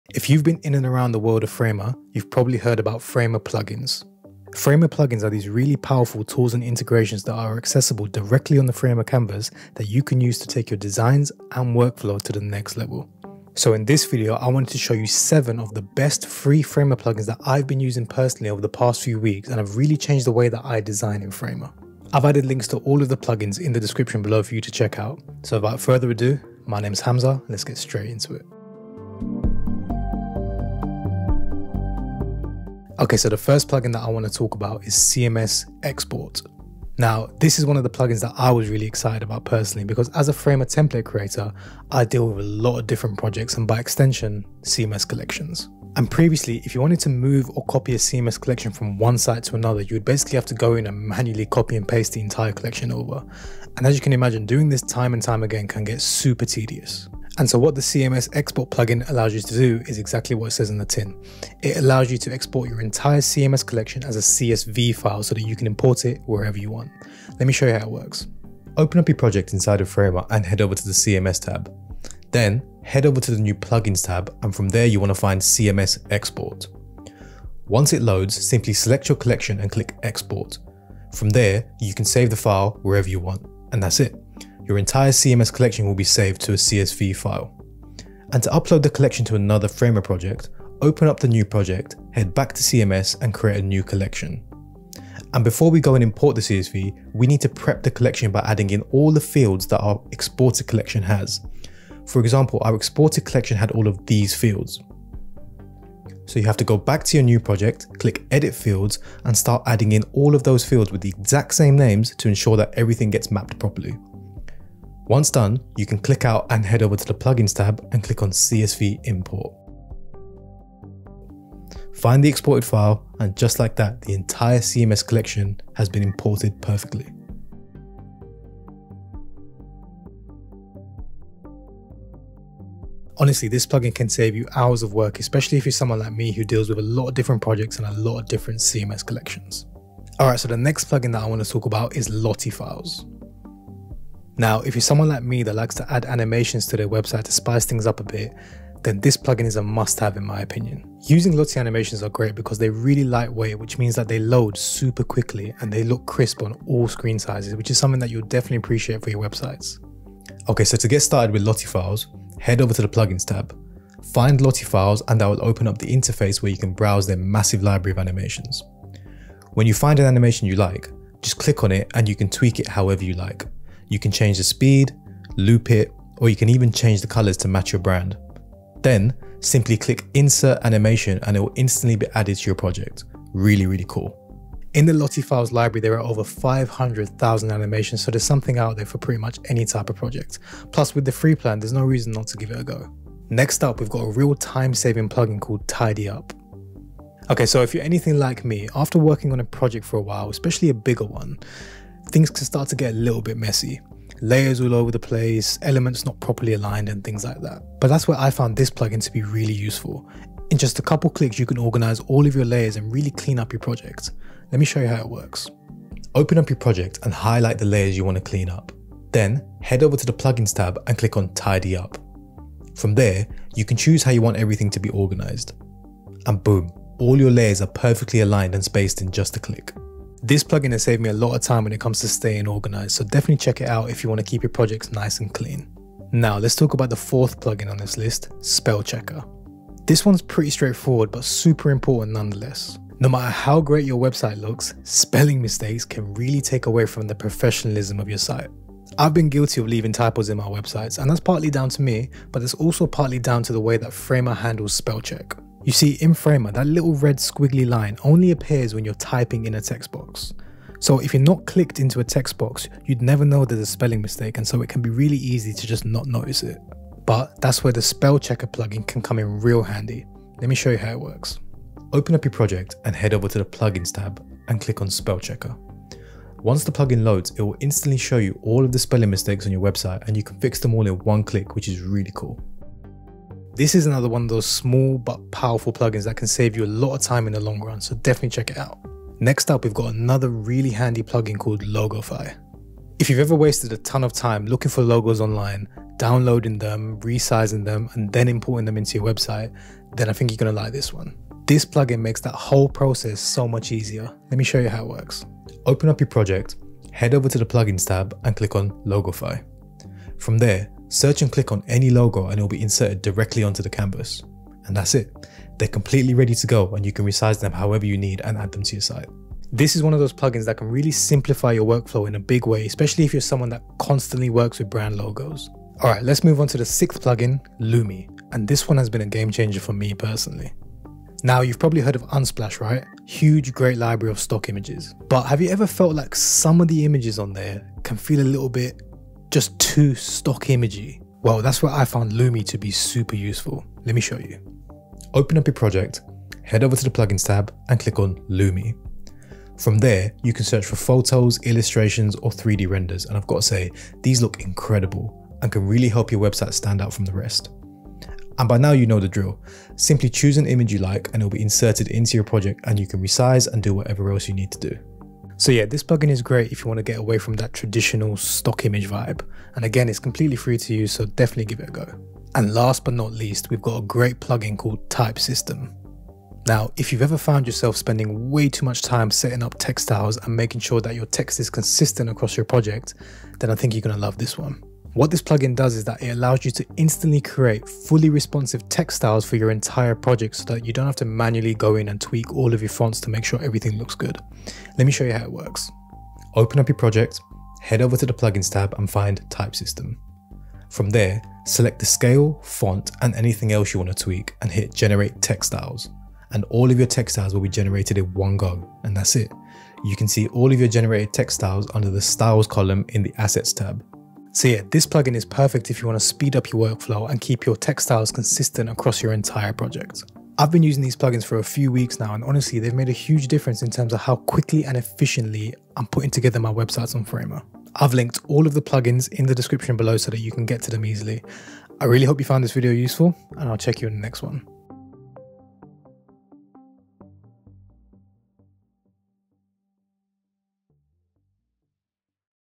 The 7 Best FREE Framer Plugins In 2024
In this video, I’ll walk you through seven of the best free Framer plugins that will streamline your design workflow and take your projects to the next level.

Chapters:

0:00 - Introduction
0:32 - Plugin 1
2:20 - Plugin 2
3:13 - Plugin 3
3:56 - Plugin 4
4:49 - Plugin 5
5:26 - Plugin 6
6:12 - Plugin 7